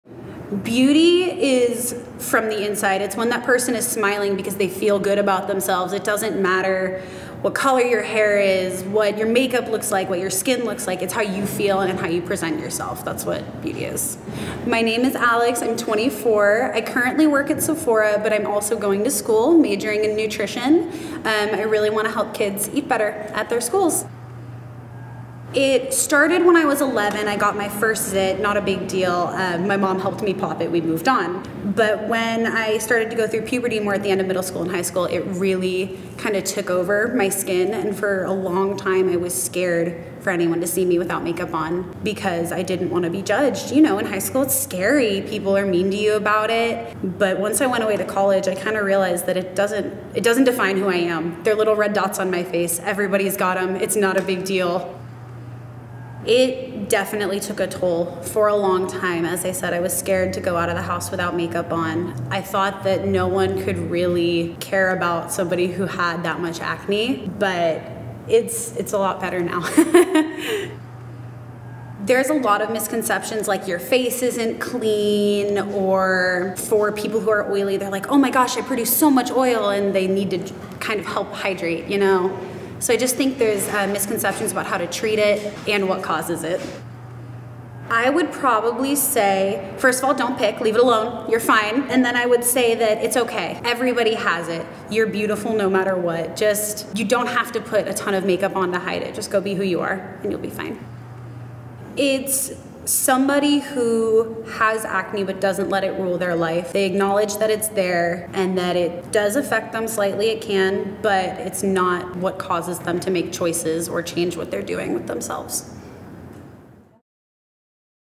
Peace Out | Acne Champs Testimonial #12: Alex
Get to know our Acne Champs… this week it our final champ in the series, Alex!

My name is Alex Zwirner and I’m 24 years old. I’m a major makeup and skincare enthusiast. I absolutely love going on adventures and trying new things!

When we say we are building a positive skin community, we mean it.

We interviewed 12 people from ages 17 - 30, across all demographics, genders, and sexuality about their journey with acne.

They opened up and vulnerably shared with us what being an acnechamp​ means to them. We’re highlighting their stories and hope you love them just as much as we do.

Ps...Acne Champs is Clean, Vegan, Gluten-Free and Cruelty Free